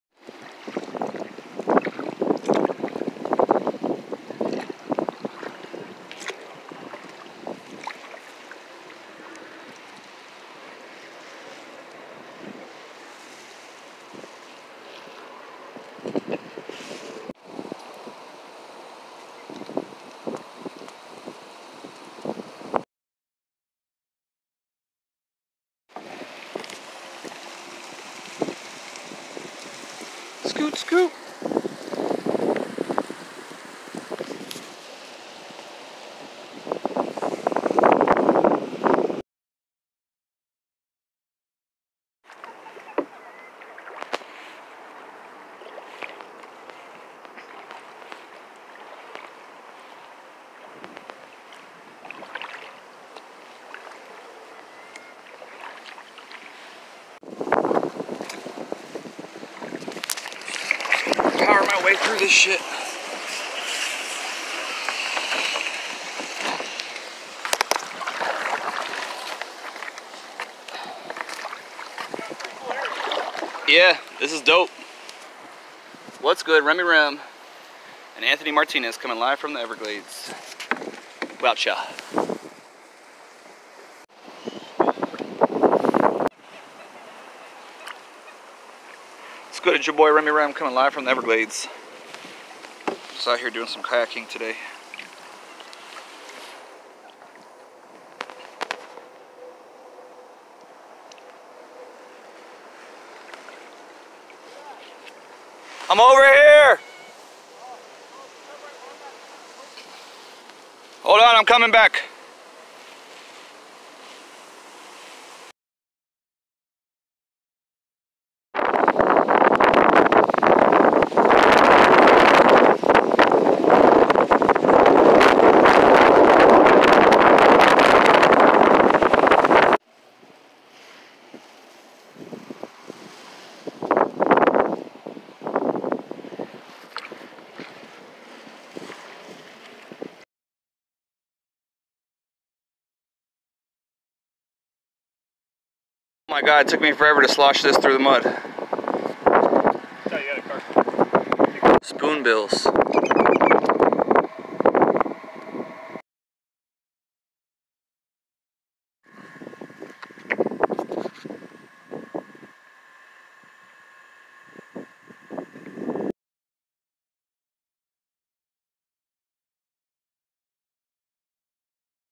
Kayak fishing A1 flow equilization basin
Kayak fishing with my buddy Anthony in the everglades. An area known as the a1flowequilizationbasin